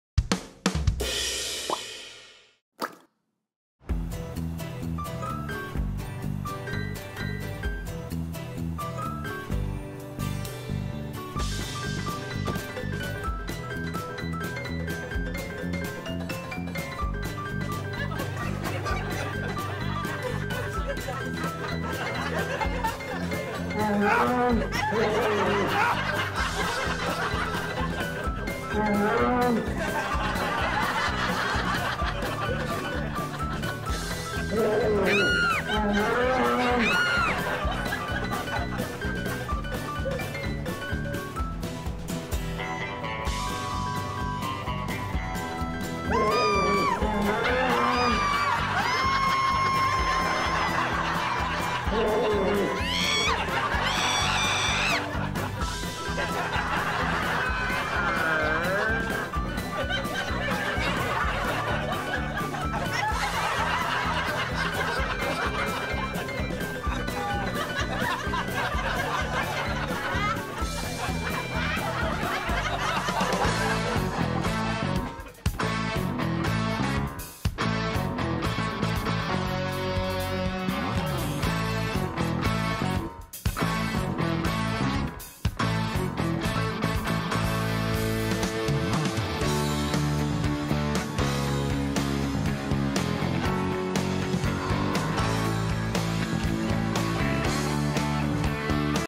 Victims Accidentally Wake Up Wild Bear
I'm torn. This majestic creature deserves its beauty sleep, but these people also deserve not to be eaten. Let us know whose side you're on!

JustforLaughs Gags Across the Internet